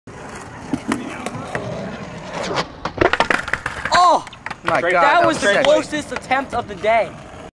CLOSEST ATTEMPT OF THE DAY!
ramped slow mo of a tre flip... well a close attempt!
ahaha good times. (old footy)